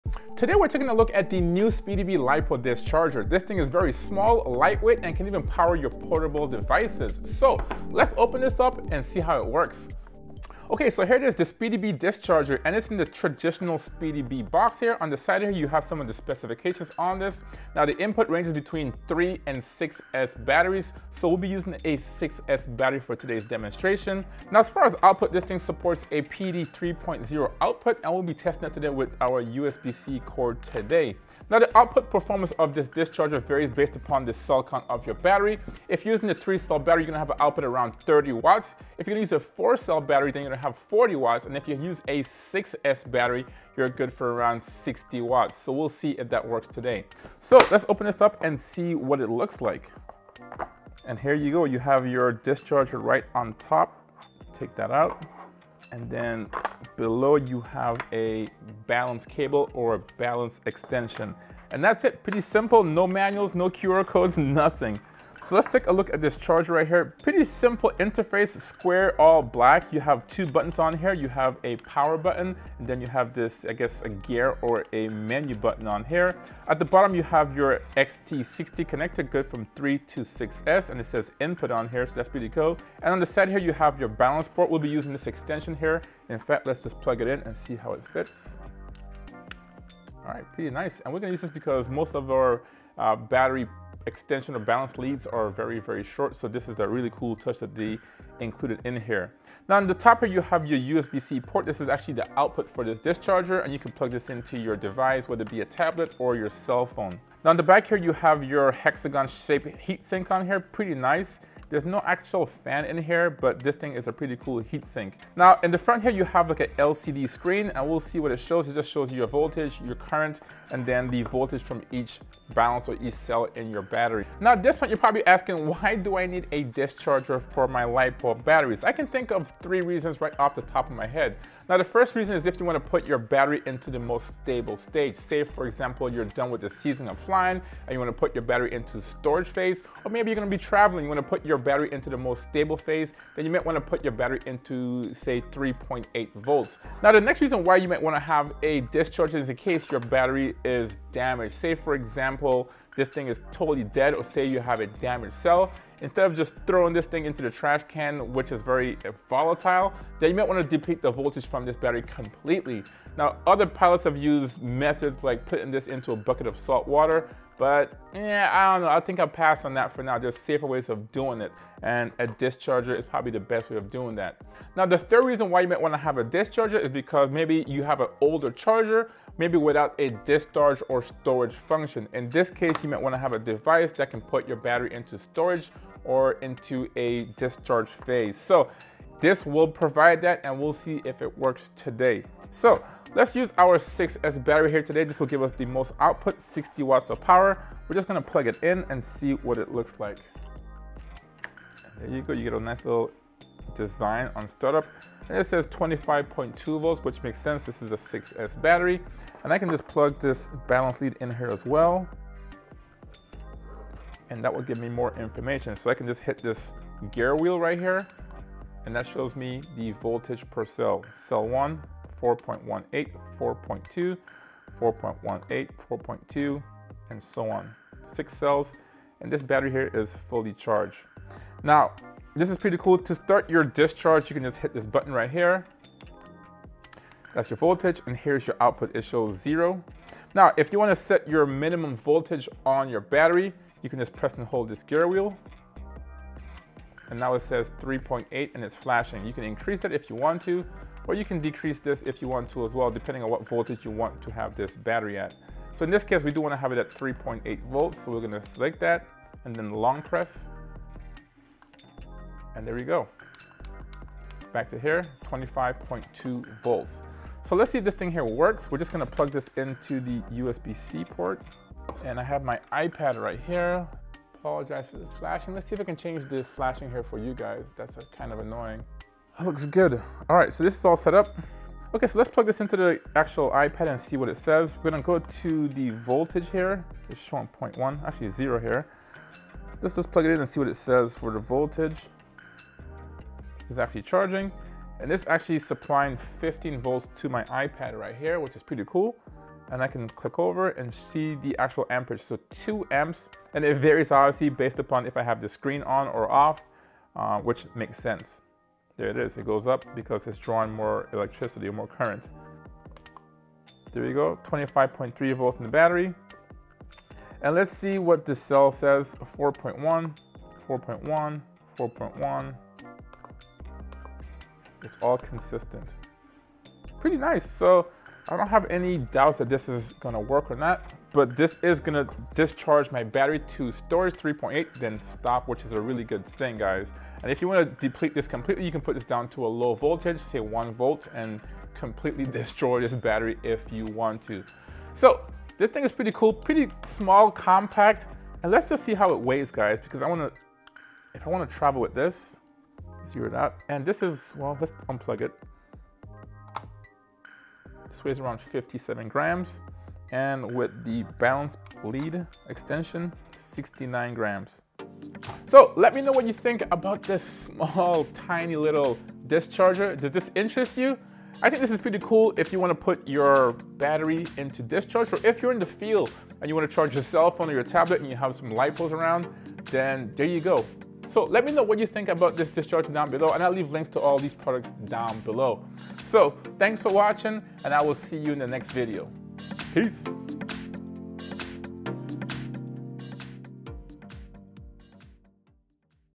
SpeedyBee Discharger | Light, Powerful, and Charges your Portable Devices
Keeping your battery at optimum performance requires maintainable such as setting their voltage to 3.8 volts.  There are numerous way to do this, but the safest is with a battery Discharger.   Today we’ll be testing the new SpeedyBee Lipo battery Discharger, and seeing if its right for you.